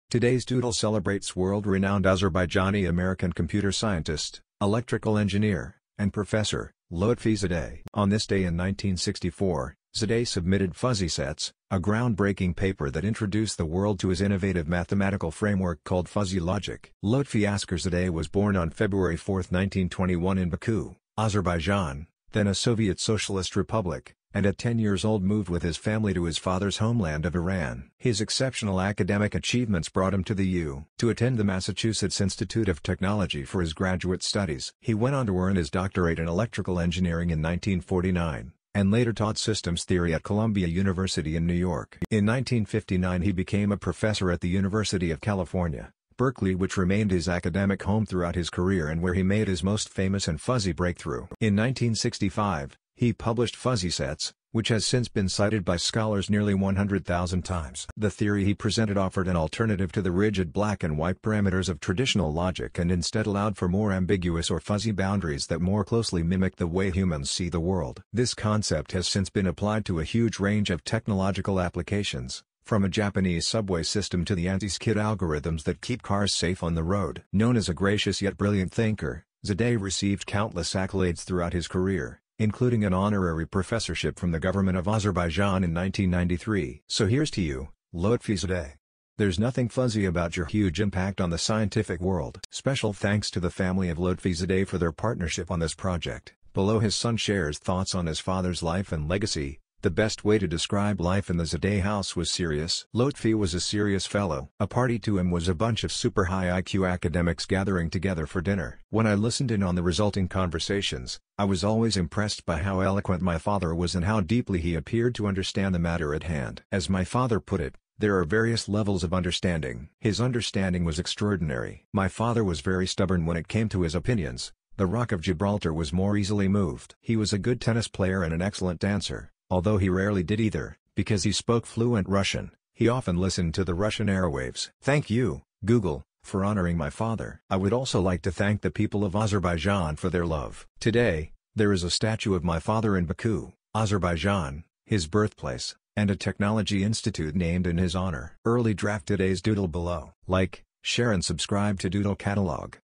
Лютфи Заде | День Лютфи Заде
Google Doodle :
День Лютфи Заде

Лютфи Заде

Celebrating Lotfi Zadeh

celebrating-lotfi-zadeh

Лютфи Заде

День Лютфи Заде

Today’s Doodle celebrates world-renowned Azerbaijani-American computer scientist, electrical engineer, and professor, Lotfi Zadeh  On this day in 1964, Zadeh submitted “Fuzzy Sets,” a groundbreaking paper that introduced the world to his innovative mathematical framework called “fuzzy logic ”Lotfi Asker Zadeh was born on February 4, 1921 in Baku, Azerbaijan (then a Soviet Socialist republic), and at 10 years old moved with his family to his father’s homeland of Iran  His exceptional academic achievements brought him to the U  to attend the Massachusetts Institute of Technology for his graduate studies  He went on to earn his doctorate in electrical engineering in 1949, and later taught  systems theory at Columbia University in New York  In 1959 he became a professor at the University of California, Berkeley–which remained his academic home throughout his career and where he made his most famous and fuzzy breakthrough In 1965, he published “Fuzzy Sets,” which has since been cited by scholars nearly 100,000 times  The theory he presented offered an alternative to the rigid “black and white” parameters of traditional logic and instead allowed for more ambiguous or “fuzzy” boundaries that more closely mimic the way humans see the world  This concept has since been applied to a huge range of technological applications—from a Japanese subway system to the anti-skid algorithms that keep cars safe on the road  Known as a gracious yet brilliant thinker, Zadeh received countless accolades throughout his career, including an honorary professorship from the government of Azerbaijan in 1993  So here’s to you, Lotfi Zadeh! There’s nothing fuzzy about your huge impact on the scientific world   Special thanks to the family of Lotfi Zadeh for their partnership on this project  Below his son shares thoughts on his father’s life and legacy:  The best way to describe life in the Zadeh house was "serious " Lotfi was a serious fellow  A "party" to him was a bunch of super high-IQ academics gathering together for dinner  When I listened in on the resulting conversations, I was always impressed by how eloquent my father was and how deeply he appeared to understand the matter at hand  As my father put it, "there are various levels of understanding " His understanding was extraordinary  My father was very stubborn when it came to his opinions—the Rock of Gibraltar was more easily moved  He was a good tennis player and an excellent dancer, although he rarely did either  Because he spoke fluent Russian, he often listened to the Russian airwaves   Thank you, Google, for honoring my father  I would also like to thank the people of Azerbaijan for their love  Today, there is a statue of my father in Baku, Azerbaijan (his birthplace), and a technology institute named in his honor   Early draft today's Doodle below   Like, Share and Subscribe to Doodle Catalog